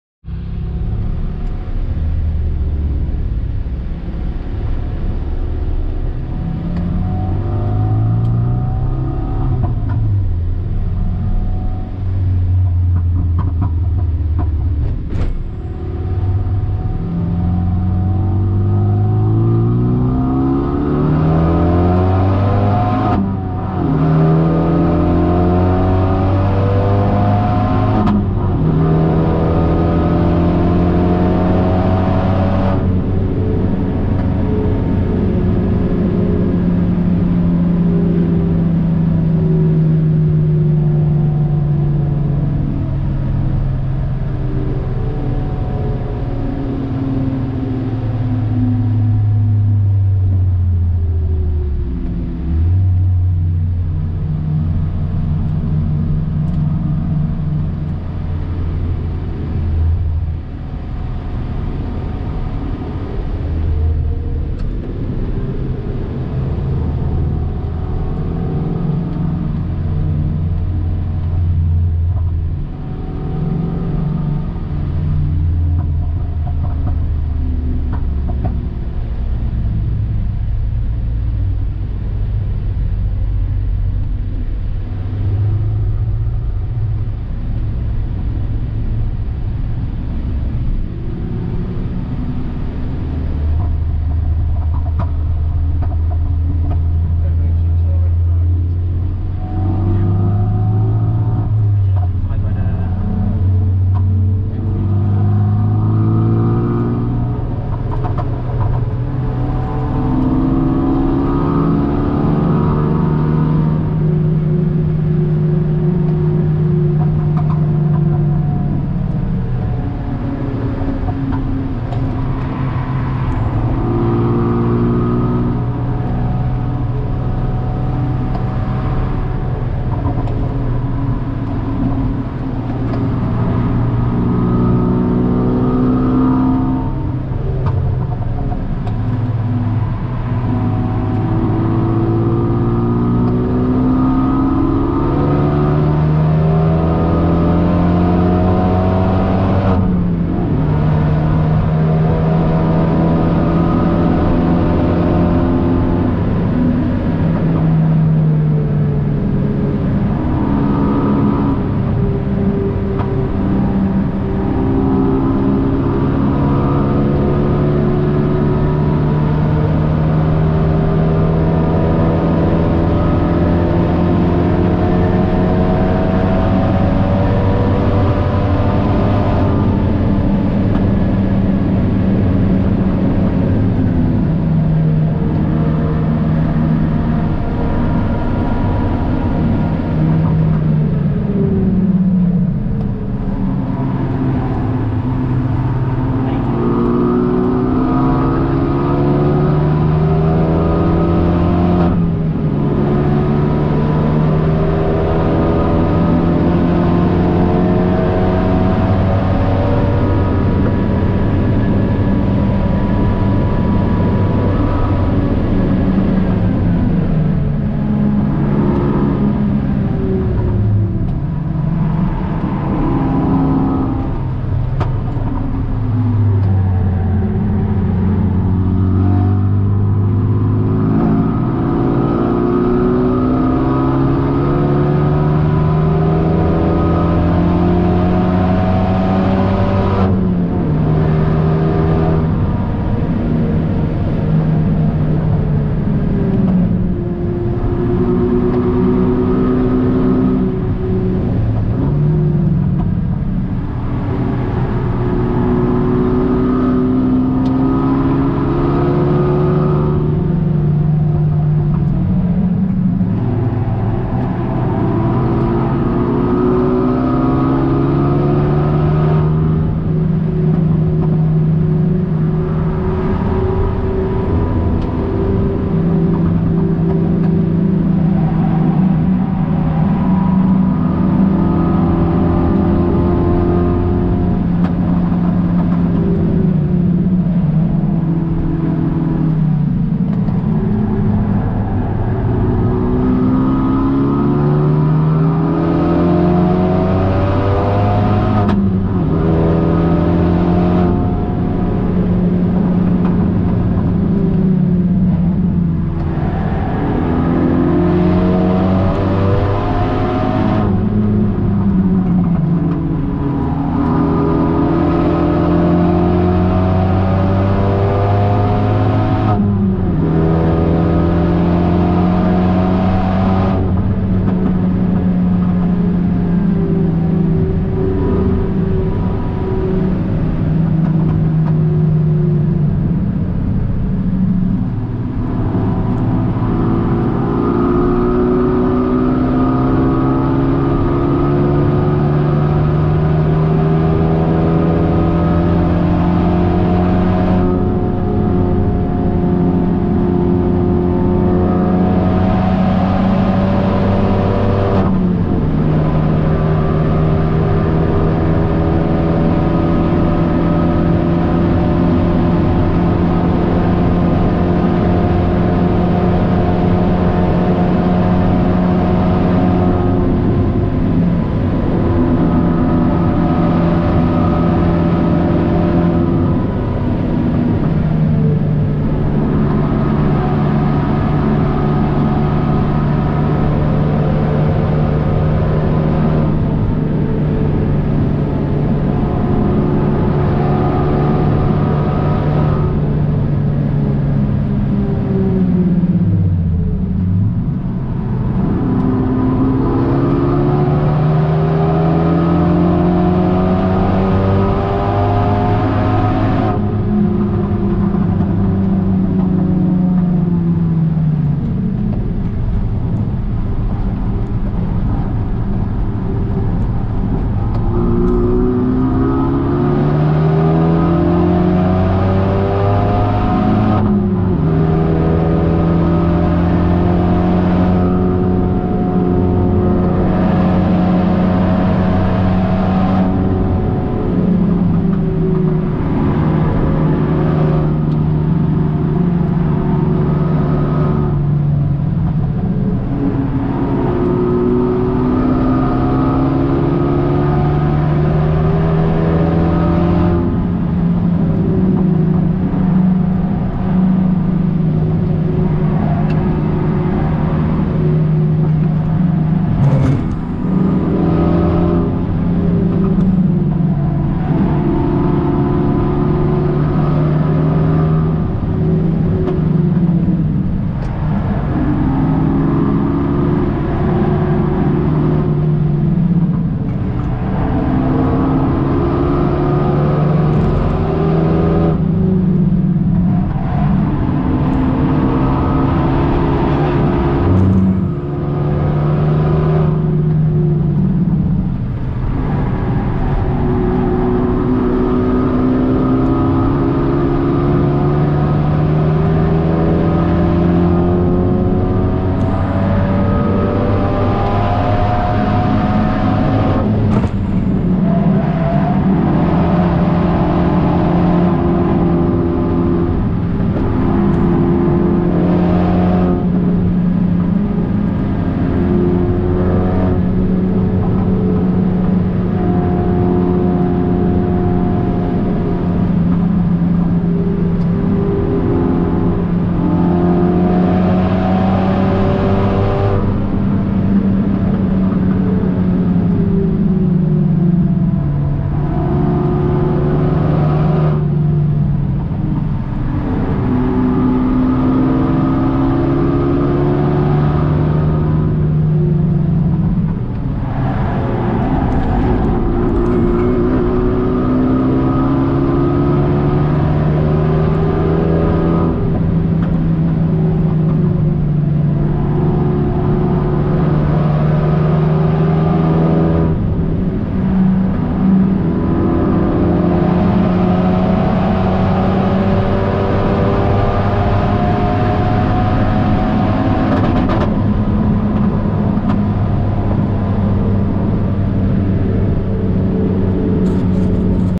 Nurburgring Touristenfahrten 01-09-24' Lap 3/6 Megane 3RS Trophy 9:15 btg W/ yellow
Lap 3 of 6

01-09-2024

BTG

- megane 3 RS 275 Trophy
- öhlins W/ Madeno Racing Setup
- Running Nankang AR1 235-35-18
- Around 289HP and 396NM